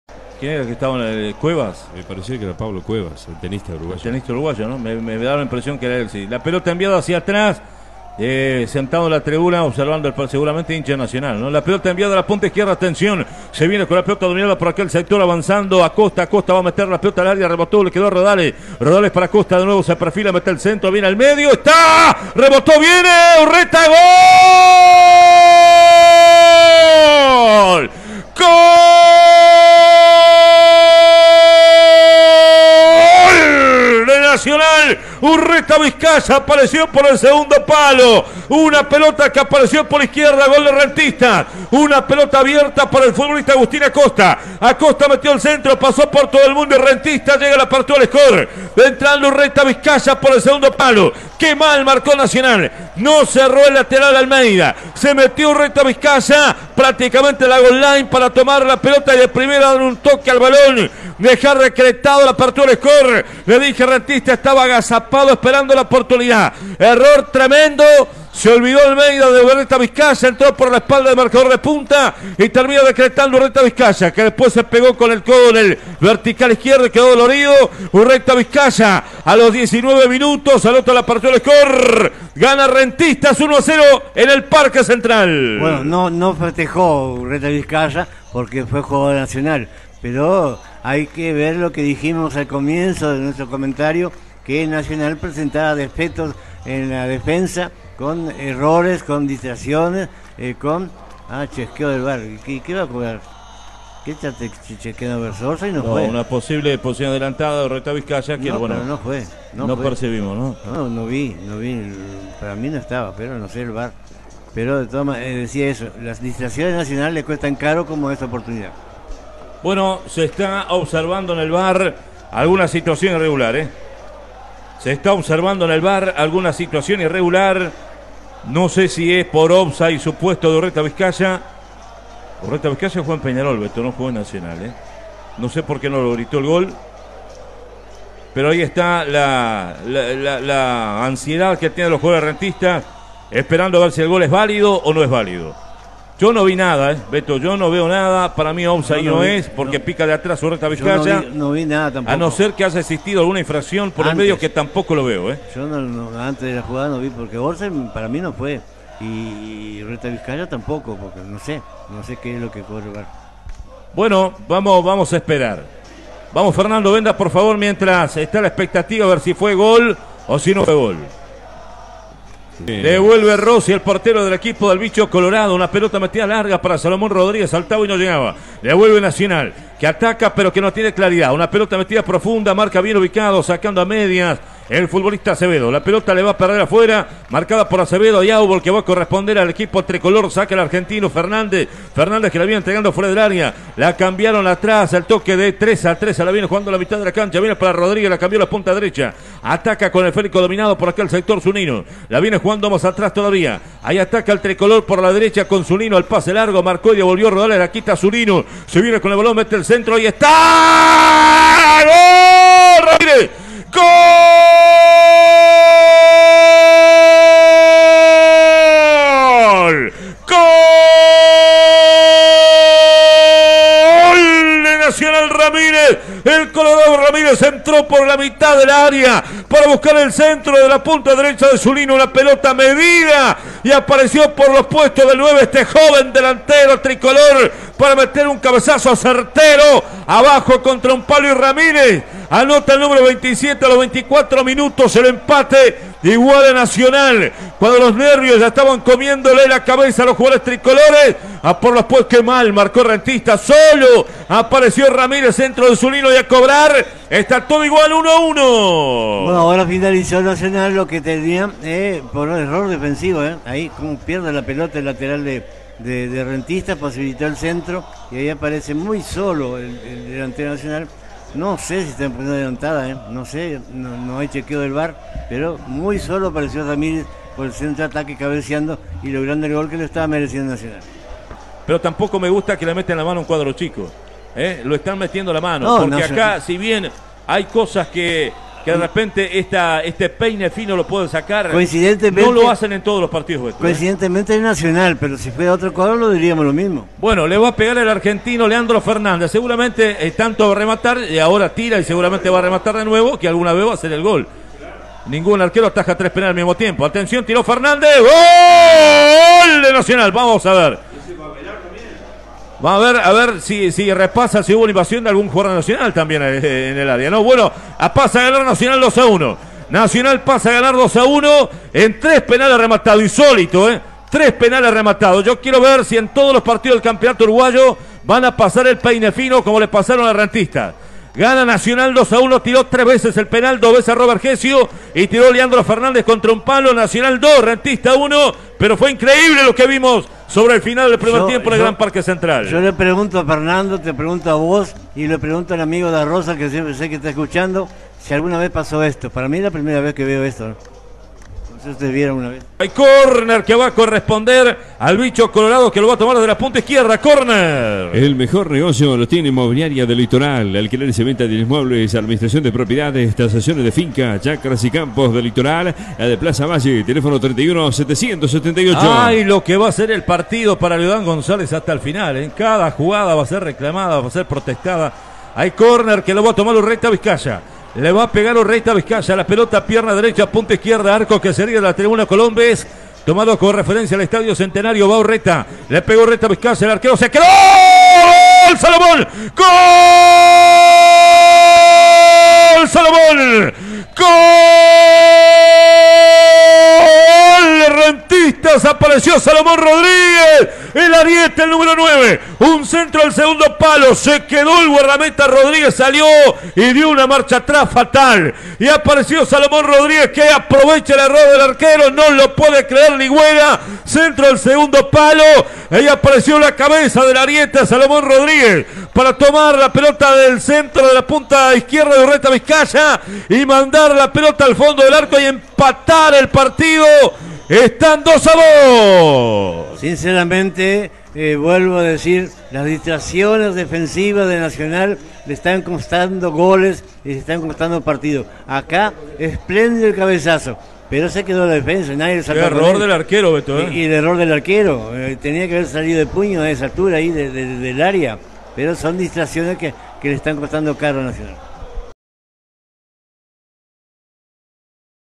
Nacional 2 Rentistas 2 - Clausura 2021 (por Germán Lecueder)
Los goles de lpartido, por Germán Lecueder para La Radio Con Vos desde Artigas.